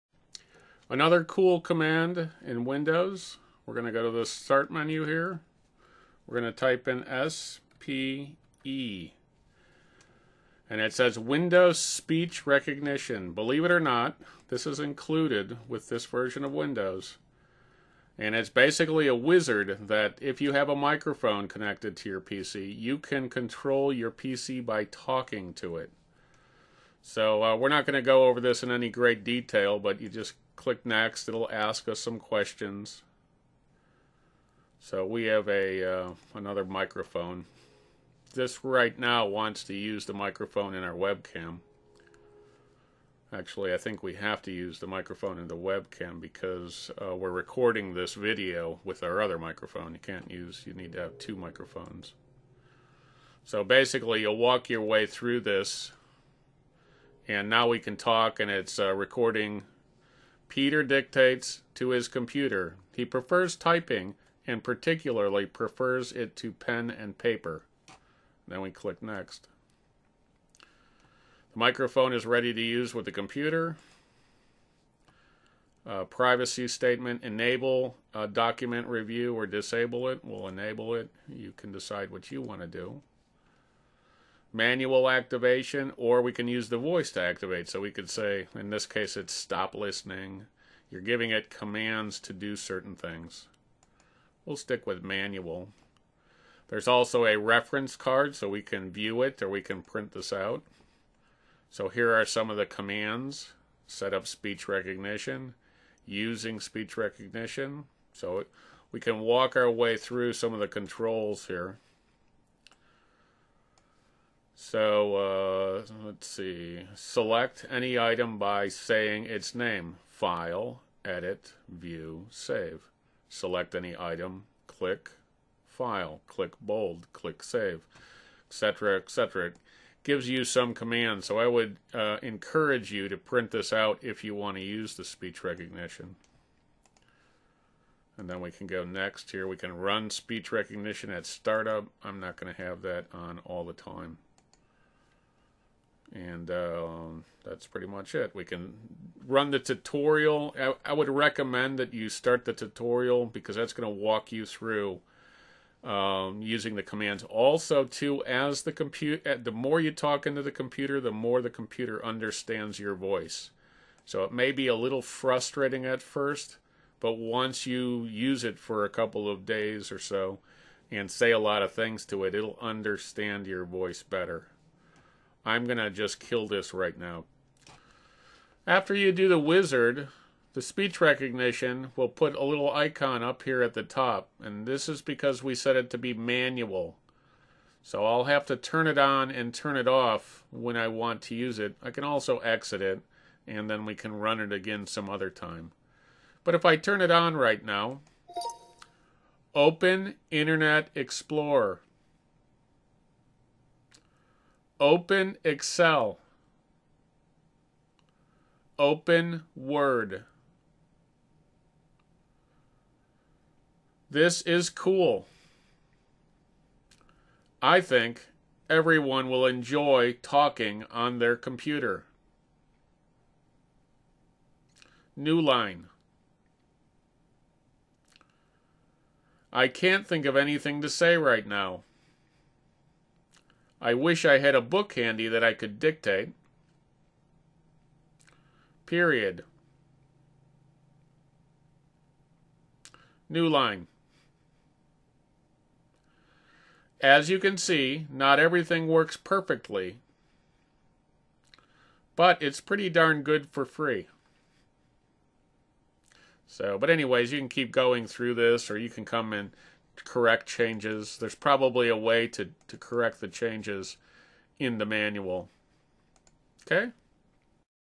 Learn Windows 8 Speech Recognition - Talk to your computer and it types for you.
Buy 5.75 hrs of Windows 8 training 77 Lessons, 308 topics $9.95

Windows 8 Speech Recognition.  Your computer can learn your voice with training and type what you say, you can run commands and other things.  You will need to practice and over time it will get better.